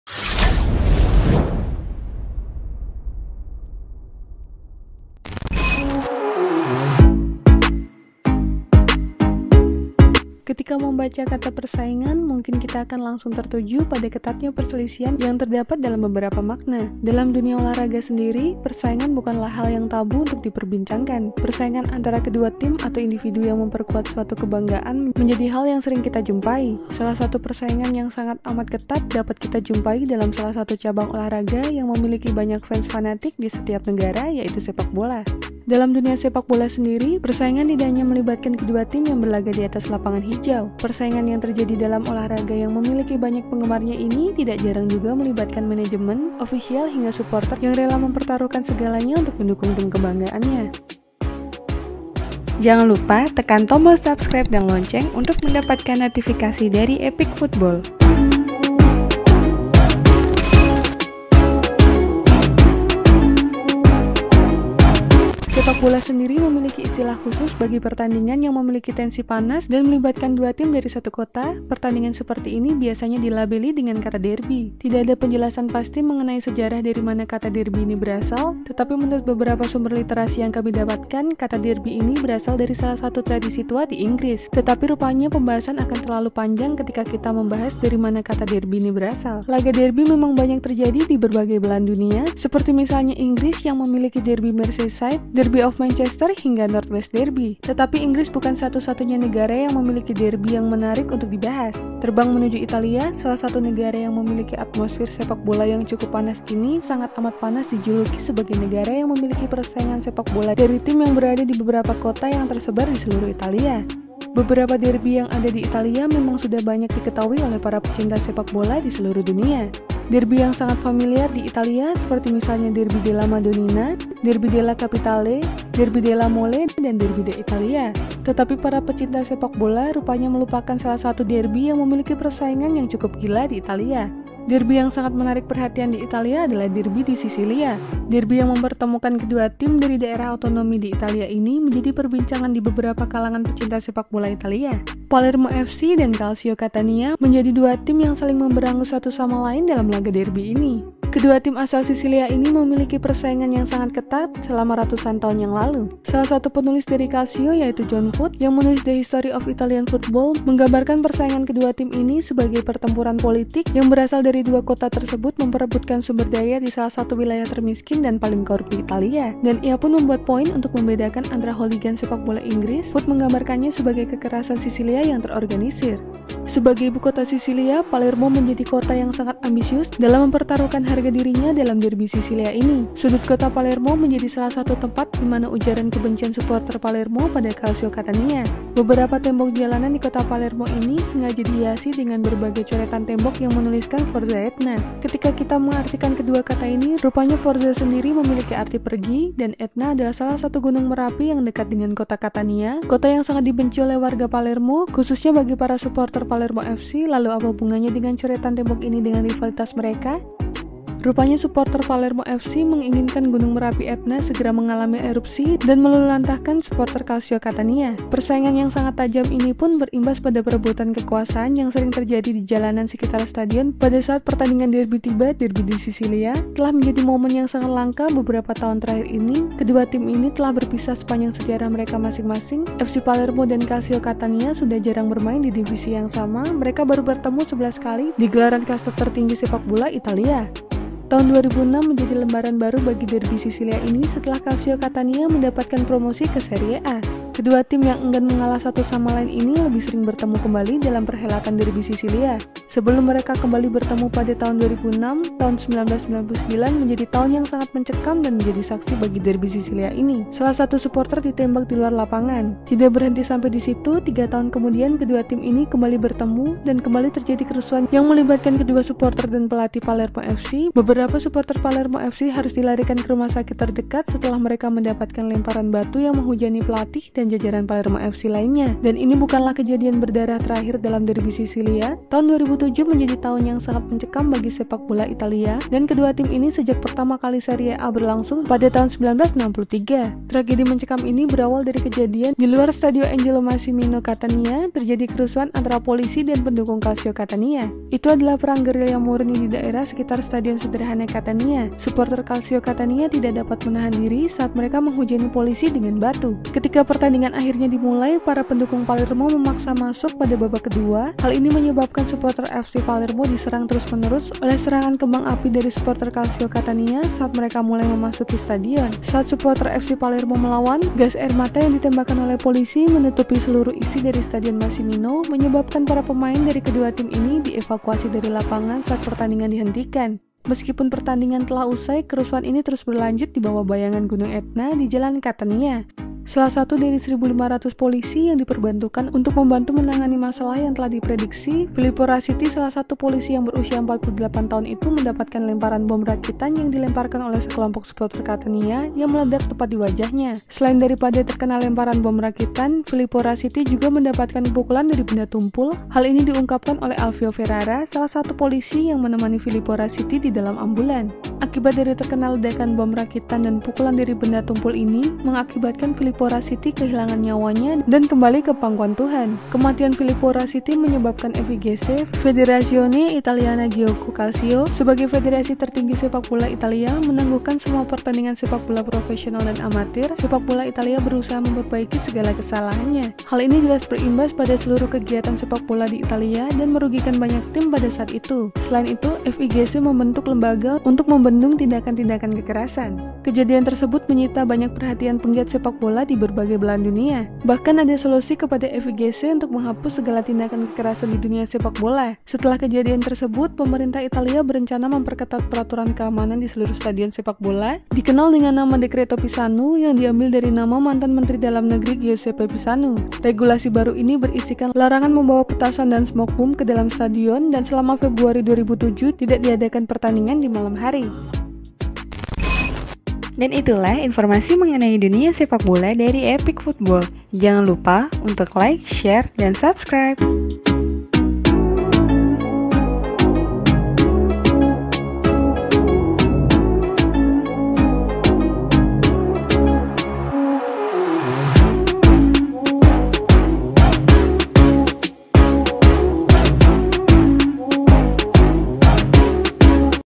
Derby di Sicilia = Palermo vs Catania ! Sisilia Memiliki Sektarianisme Tak Terbantahkan.
Dalam dunia sepak bola sendiri persaingan tidak hanya melibatkan kedua tim yang berlaga diatas lapangan hijau. Persaingan yang terjadi dalam olah raga yang memiliki banyak penggemarnya ini tidak jarang juga melibatkan manajemen, ofisial hingga suporter yang rela mempertaruhkan segalanya untuk mendukung tim kebangaannya.
Sepak bola sendiri memiliki istilah khusus bagi pertandingan yang memiliki tensi panas dan melibatkan dua tim dari satu kota, pertandingan seperti ini biasanya dilabeli dengan kata “derby”. Tidak ada penjelasan pasti mengenai sejarah darimana kata derby ini berasal, tetapi menurut beberapa sumber literasi yang kami dapatkan, kata derby ini berasal dari salah satu tradisi tua di Inggris. Tetapi rupanya pembahasan akan terlalu panjang ketika kita membahas darimana kata derby ini berasal.
Laga derby memang banyak terjadi diberbagai belahan dunia, seperti misalnya Inggris yang memiliki Derby Merseyside, Derby of Manchester hingga North-West Derby. Tetapi Inggris bukan satu-satunya negara yang memiliki derby yang menarik untuk dibahas. Terbang menuju Italia, salah satu negara yang memiliki atmosfir sepak bola yang cukup panas ini sangat amat pantas dijuluki sebagai negara yang memiliki persaingan sepak bola dari tim yang berada dibeberapa kota yang tersebar diseluruh Italia.

Info Berita Bola Terbaru Hari ini dan Berita Bola Terkini - Berita Bola Terlengkap Hari Ini 2021